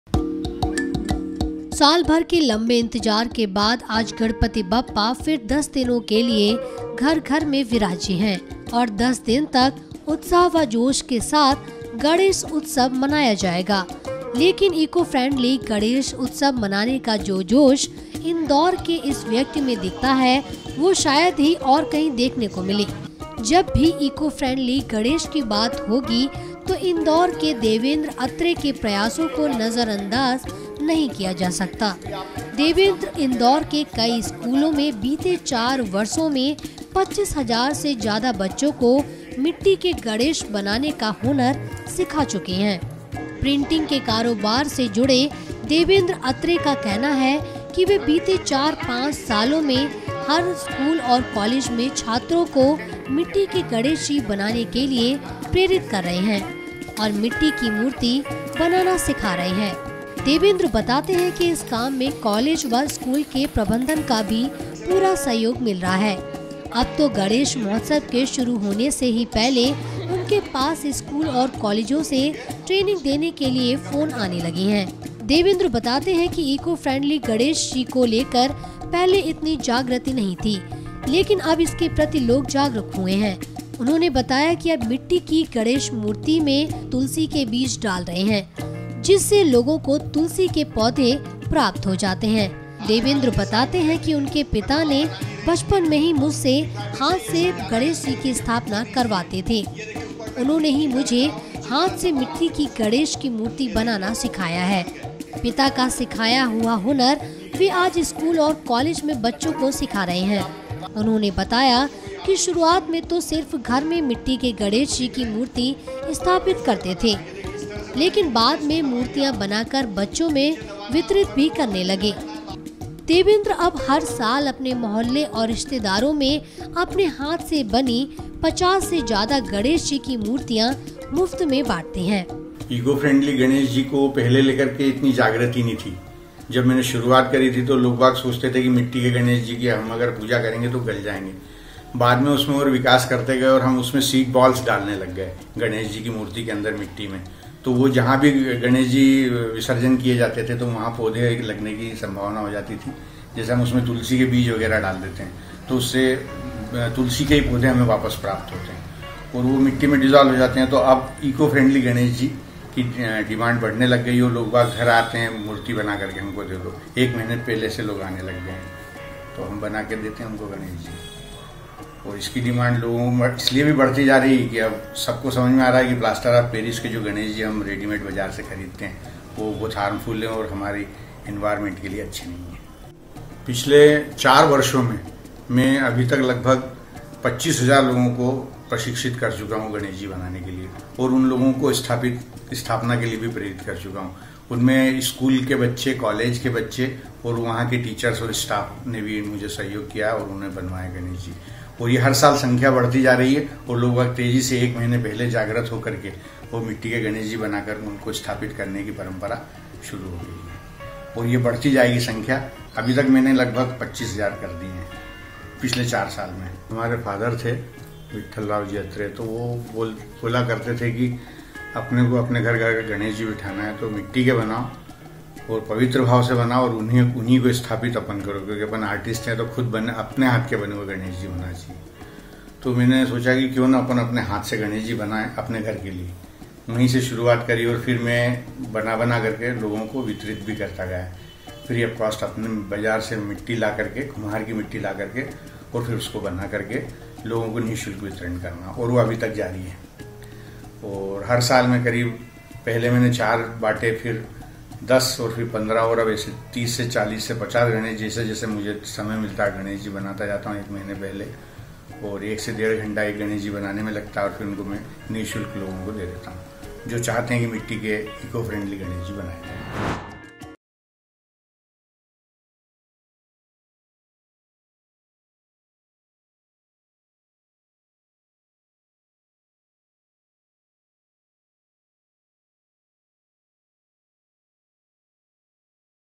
Indore के Devendra Atre ने 25,000 students को सिखाया मिट्टी के गणेश जी बनाना
सालभर के लंबे इंतजार के बाद आज गणपति बप्पा फिर 10 दिनों के लिए घर-घर में विराजेंगे और 10 दिन तक उत्साह व जोश के साथ गणेश उत्सव मनाया जाएगा, लेकिन इकोफ्रेंडली गणेश उत्सव मनाने का जो जोश इंदौर के इस शख्स में दिखता है, वो शायद और कहीं देखने को नहीं मिलेगा।